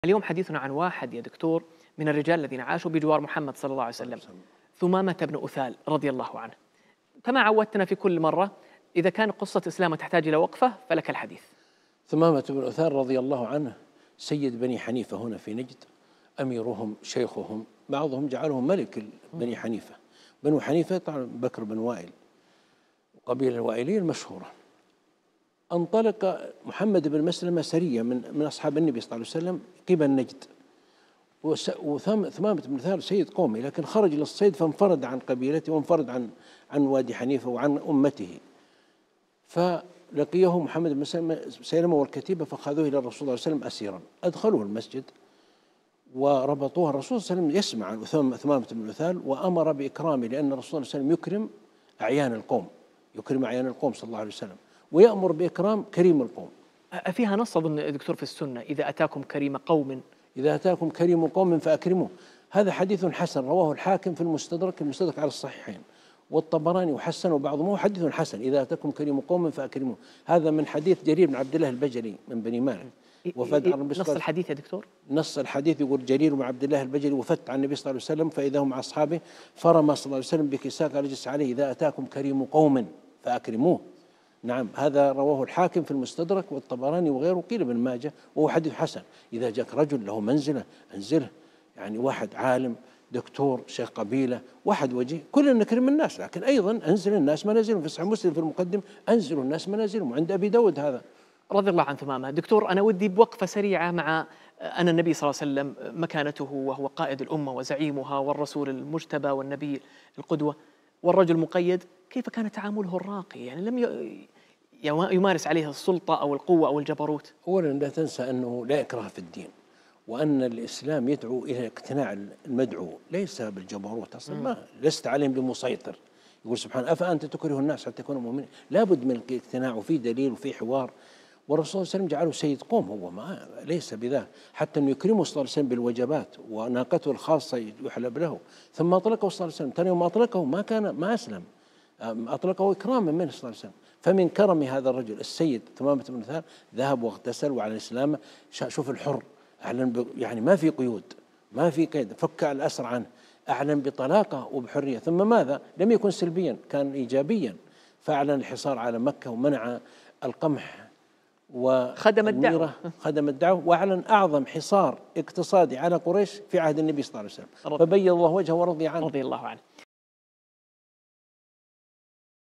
الصحابي ثمامة بن أثال - رضي الله عنه قبسات من حياته مع الدكتور عائض القرني
الصحابي ثمامة بن أثال - رضي الله عنه
قبسات من حياته مع الدكتور عائض القرني